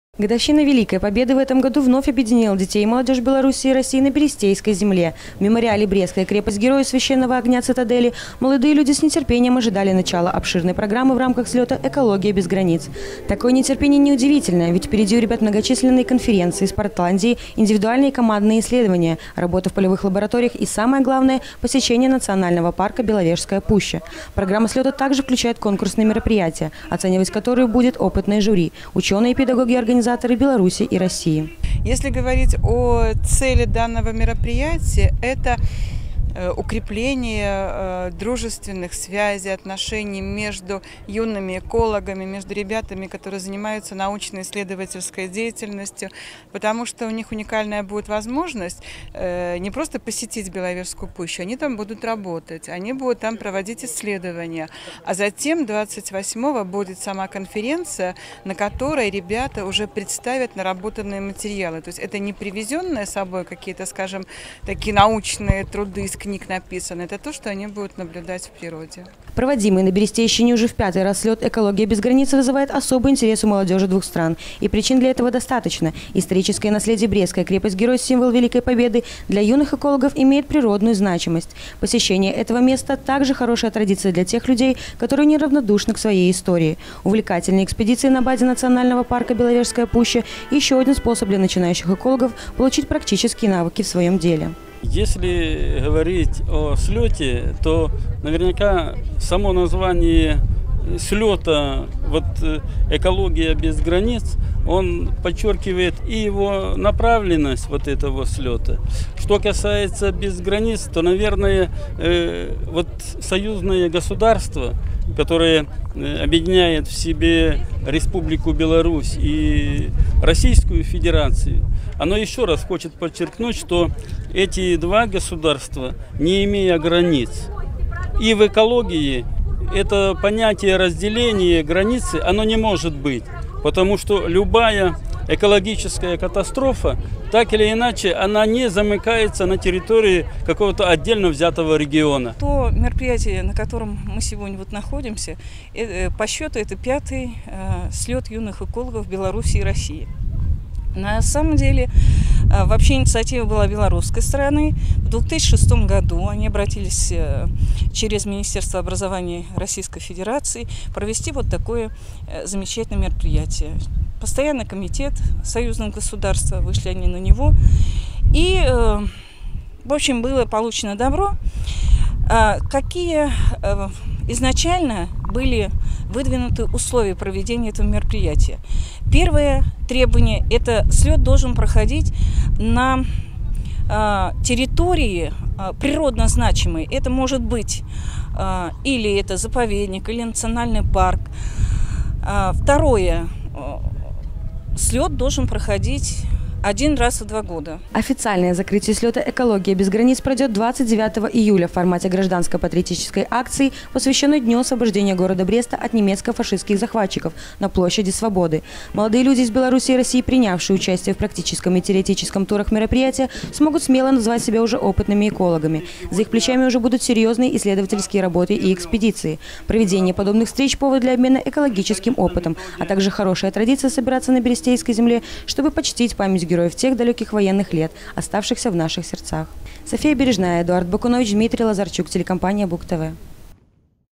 2014-07-23 г. Брест Телекомпания Буг-ТВ. Международный слет экологов.
За свою  длинную историю  неизменным для Беларуси всегда оставалась гордость за добрососедство с Россией. Две страны уже десятки лет поддерживают тесные деловые и дружеские отношения. Одним из свидетельств этого можно назвать  стартовавший 22 июля в Бресте слёт юных экологов Беларуси и России «Экология без границ», посвящённый 70-летию освобождения Беларуси от немецко-фашистских захватчиков.
Подобные акции проводятся по инициативе Министерства образования Республики Беларусь и Министерства образования и науки Российской Федерации раз в два года. Этим летом в период с 22 по 29 июля 70 юных экологов Беларуси и более десятка участников из российских регионов проведут многочисленные  исследования, а также поработают в полевых лабораториях. Начало самого слёта положил торжественный митинг-открытие, на котором побывала  наша съемочная группа.